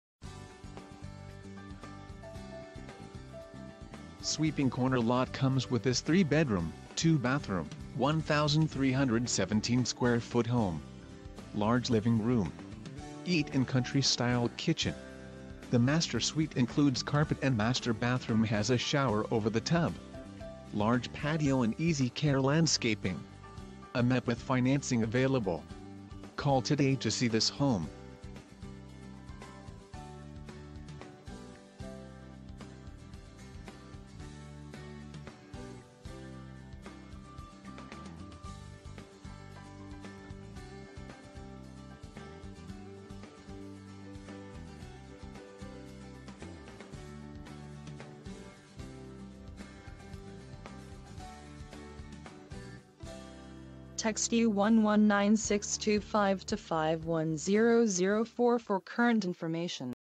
656 Livery Court Henderson NV
Sweeping corner lot comes with this 3 bedroom, 2 bathroom, 1,317 square foot home  Large living room  Eat in country style kitchen The master suite includes carpet and master bathroom has a shower over the tub  Large patio and easy care landscaping  Homepath Financing Available  Call today to see this home!

Video uploaded on Sat 23 Nov, 2013
real estate Henderson NV